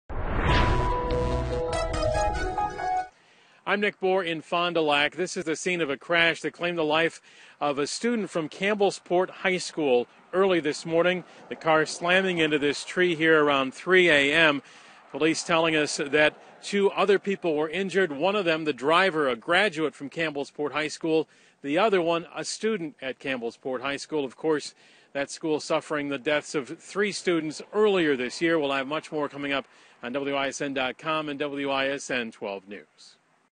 Campbellsport grieves another student's death
Campbellsport - a town already grieving from the loss of three students earlier this year - is griving again as a student was killed in an automobile accident in Fond du Lac this morning.